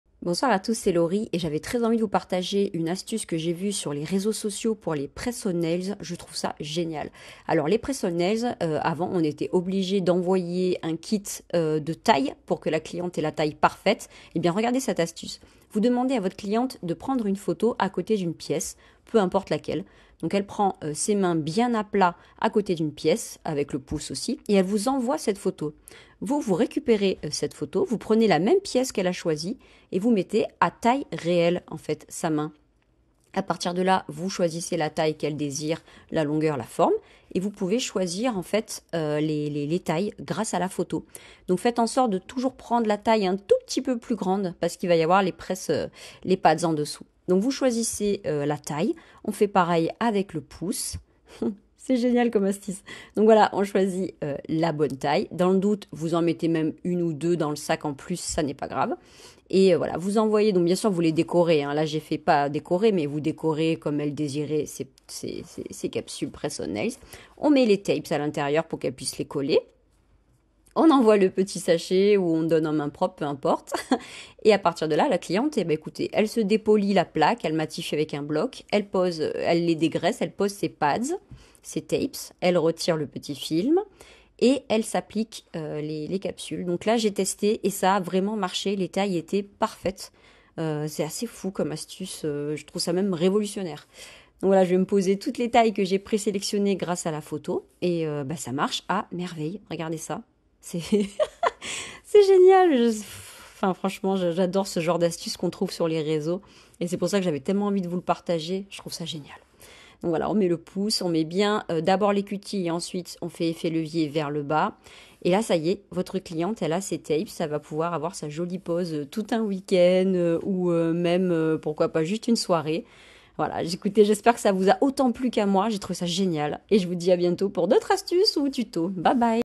M'Nails - Mardi Folie 72 : Astuce Press On
Mardi Folie 72 : Astuce Press On

Vous vendez des prestations de Press On nails et en avez marre d'envoyer des kits de taille ? 💅

Découvrez donc cette technique astucieuse pour mesurer les capsules de vos clientes à distance !

Déroulé :
- Demander une photo de la main bien à plat de vos clientes à côté d'une pièce
- Poser votre téléphone avec la photo réceptionnée près de vous
- Poser la même pièce sur l'écran et redimensionner la photo jusqu'à atteindre la taille réelle
- Mesurer les capsules. L'encoche doit recouvrir un côté et l'autre de la plaque au niveau des zones de stress. Préférez toujours une taille plus grande car les pads collants s'ajouteront.
- Décorer les capsules
- Les envoyer à la cliente avec les bandes de Tapes ainsi qu'un mini kit de préparation